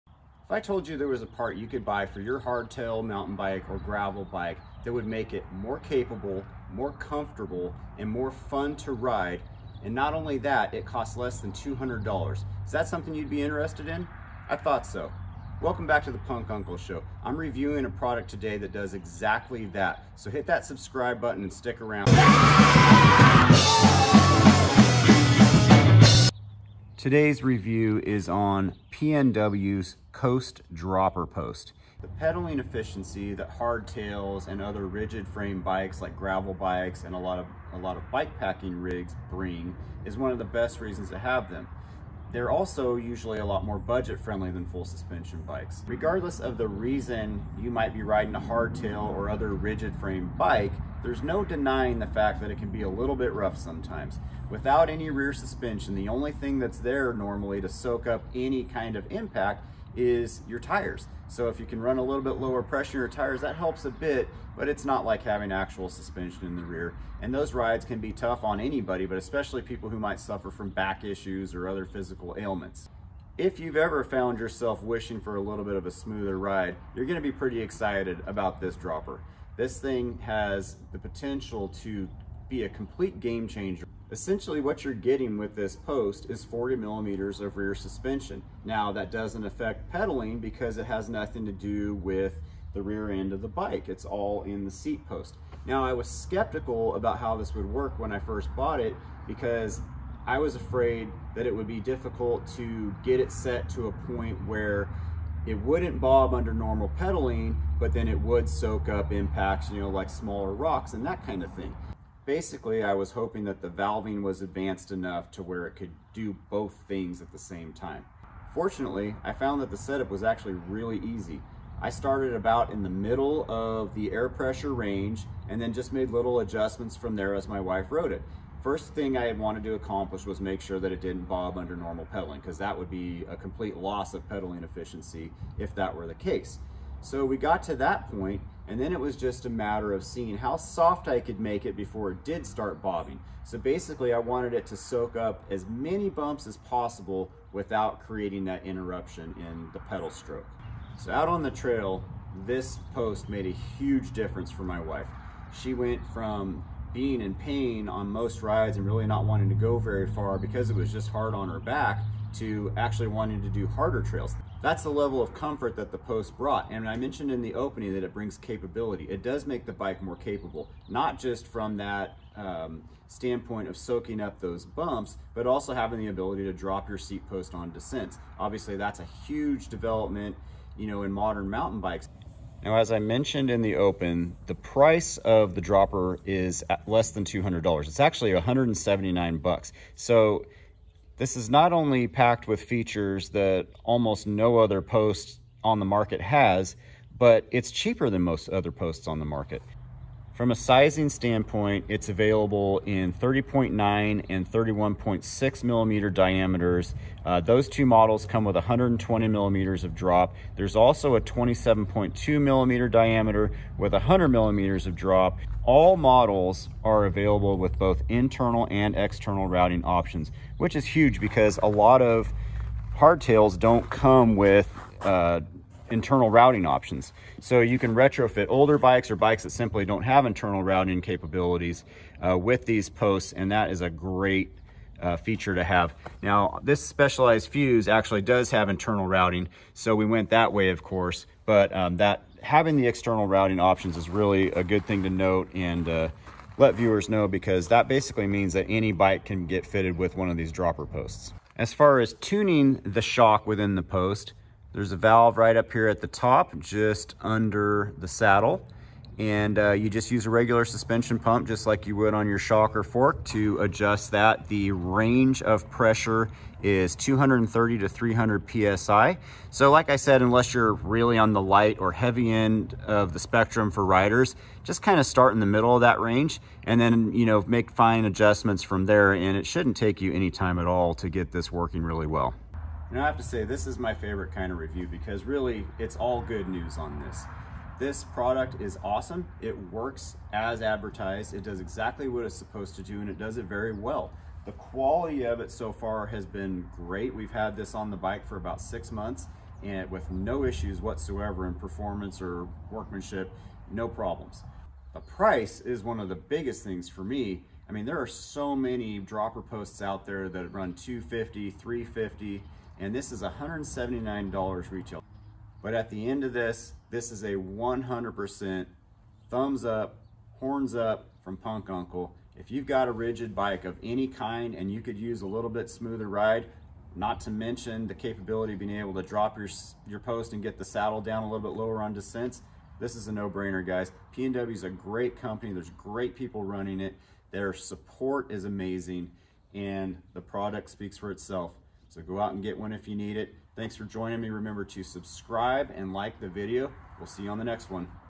PNW COMPONENTS COAST DROPPER SEAT POST REVIEW - It's a GAME CHANGER for Hardtails and Gravel bikes!
Whether you're on a hardtail mountain bike, gravel bike, bike packing setup or any other rigid framed bicycle, the ride can be punishing once the terrain starts getting bumpy. The Coast dropper seat post by PNW Components, is an innovative and affordable way to take some of the harshness out of the ride. You get 40mm of suspension in the seat post, plus the ability to drop your saddle and get it out of your way on descents. Essentially, installing this post can make your bike more capable, comfortable and fun! PNW offers 3 different diameters to fit various frames, plus internal and external cable routing options. What that means is that you can put one of these posts on almost any bike frame out there! The only question left is, whether or not it works. Watch the video and find out!